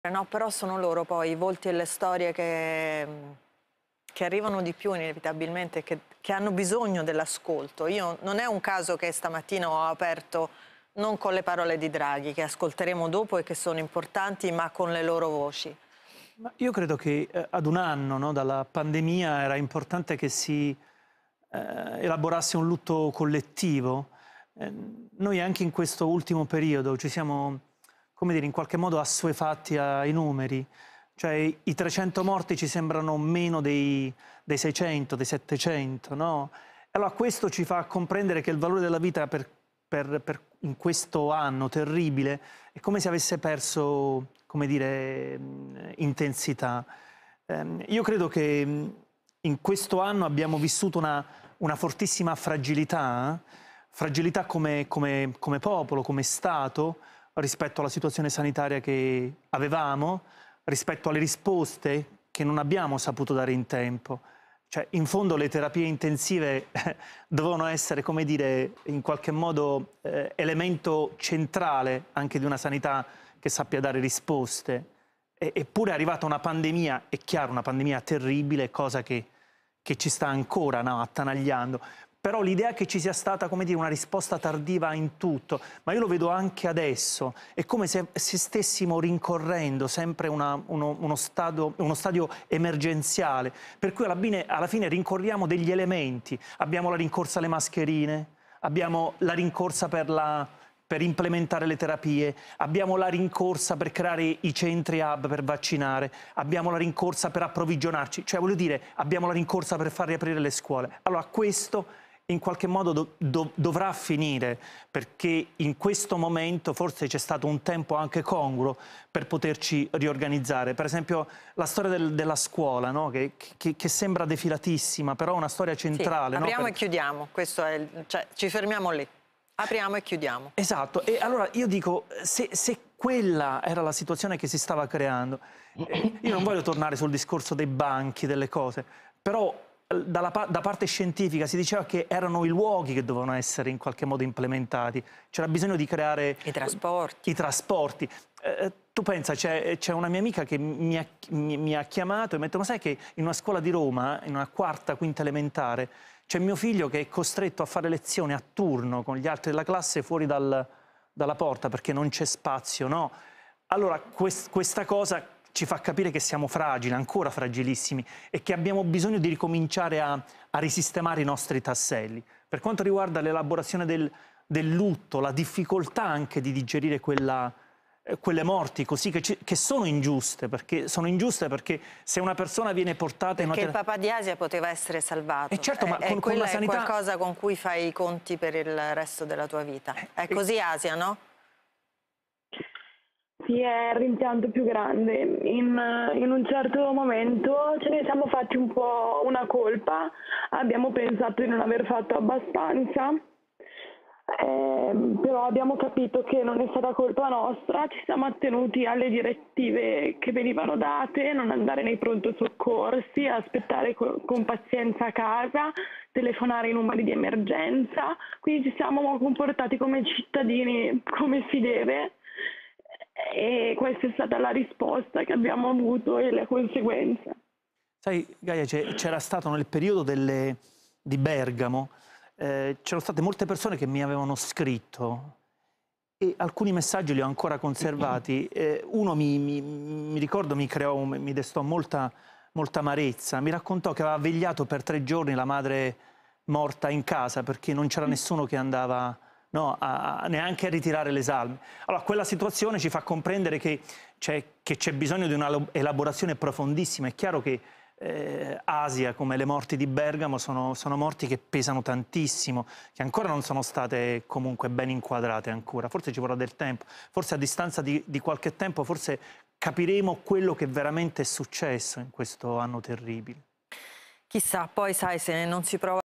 Morti Covid, Domenico Iannacone: "Dopo un anno era giusto elaborare un lutto collettivo, il ...
Il giornalista Domenico Iannacone sulla Giornata Nazionale in ricordo delle vittime del Covid: "Dopo un anno era giusto elaborare un lutto collettivo, il valore della vita sembra aver perso intensità"